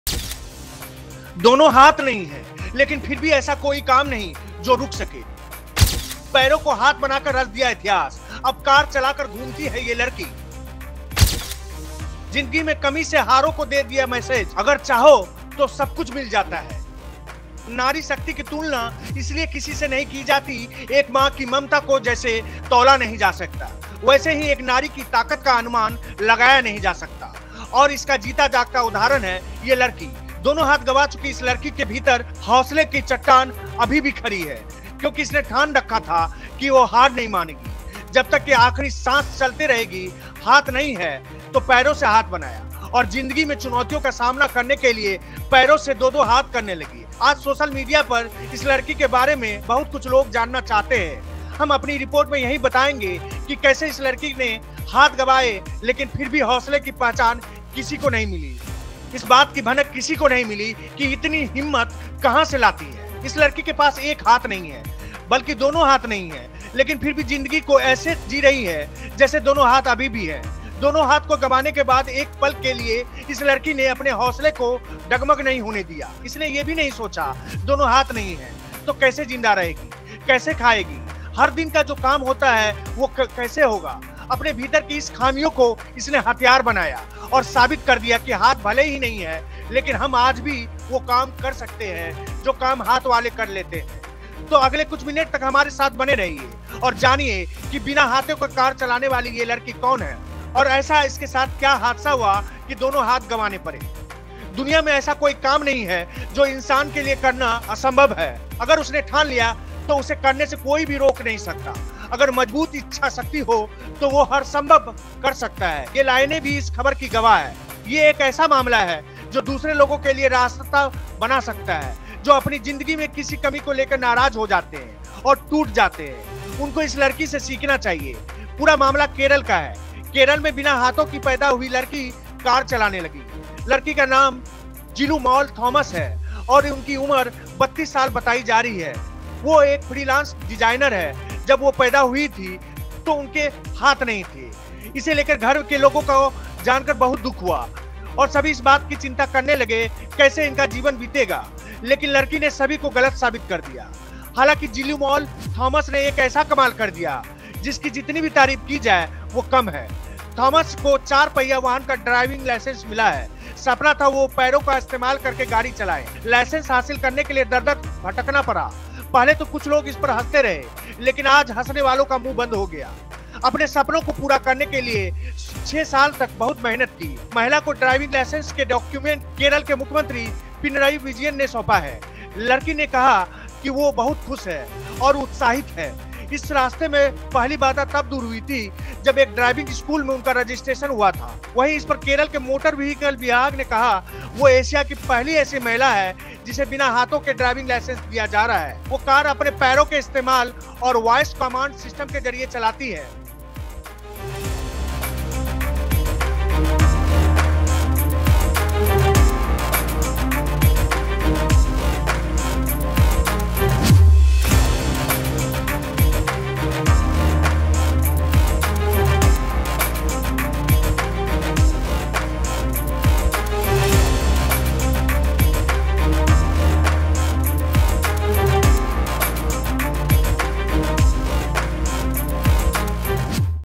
दोनों हाथ के बिना कार चलाने वाली लड़की कौन है ? | Jilumol Mariet Thomas handicap driver
दोनों हाथ के बिना कार चलाने वाली लड़की कौन है ? देखिए..सोशल मीडिया पर लोग कर रहे हौसले को सलाम...